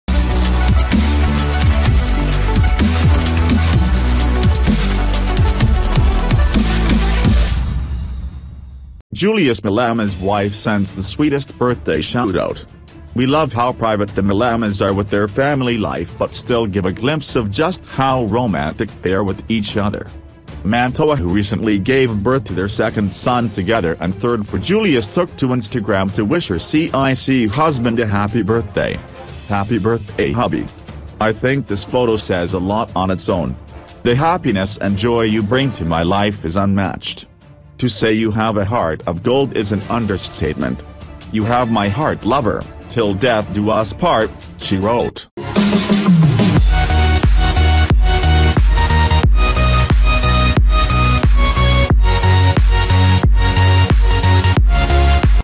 Julius Malema’s Wife Sends The Sweetest Birthday Shoutout
Julius Malema’s Wife Sends The Sweetest Birthday Shoutout! We love how private the Malemas are with their family life but still give a glimpse of just how romantic they are with each other.